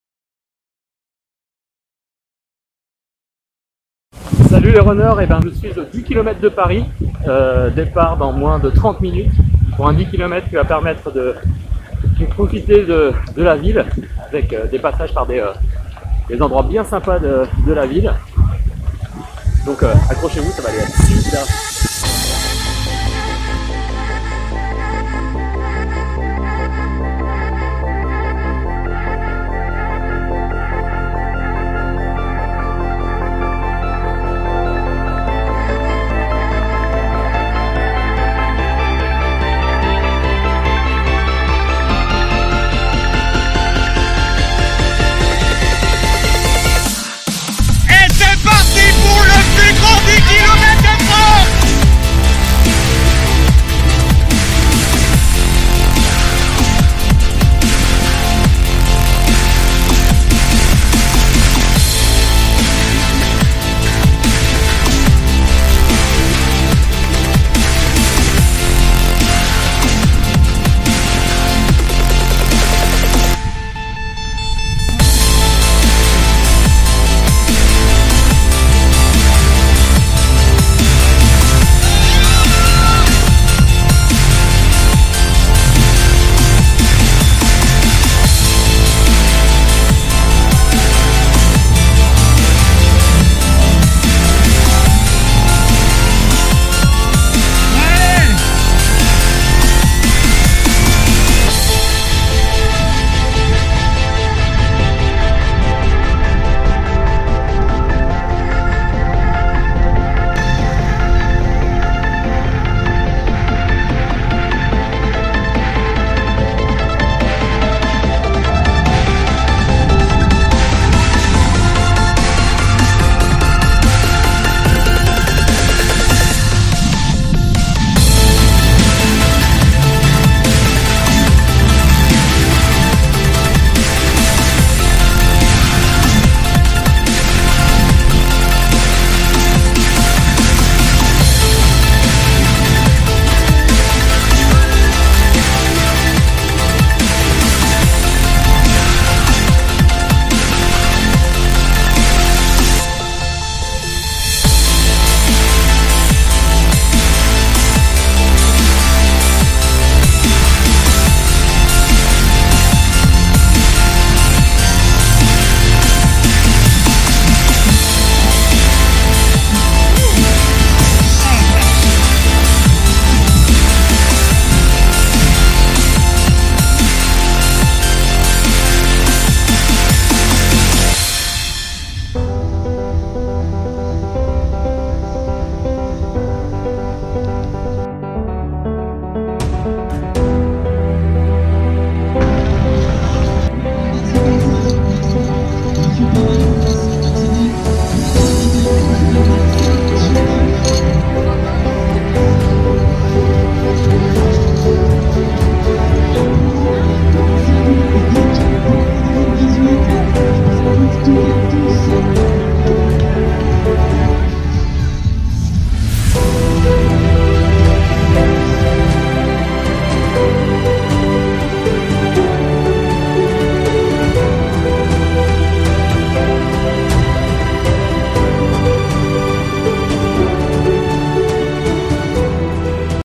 10KM de Paris 2018
VIdéo de ma course lors du 10km de Paris en juin 2018.